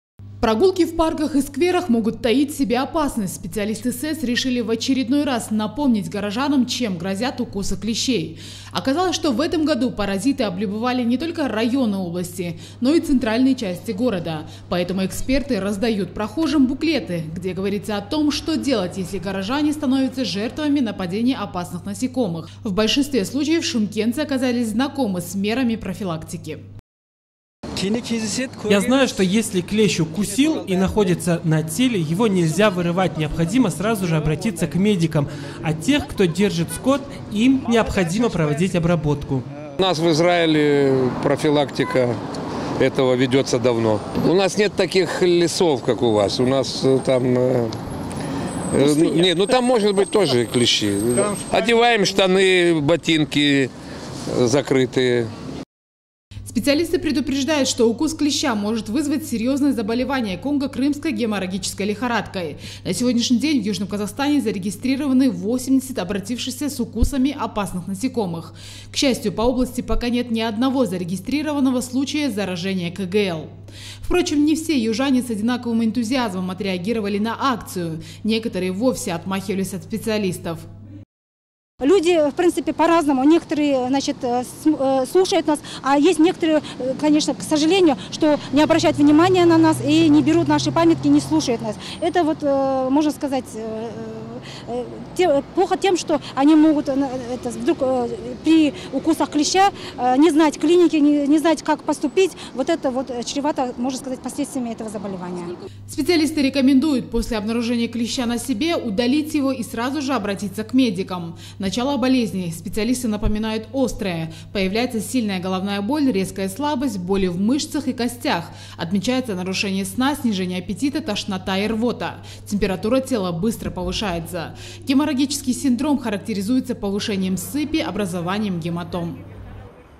Прогулки в парках и скверах Шымкента могут таить в себе опасность
Специалисты СЭС вышли на улицы города. Они напоминают горожанам, что с наступлением тепла от встречи с клещами никто не застрахован.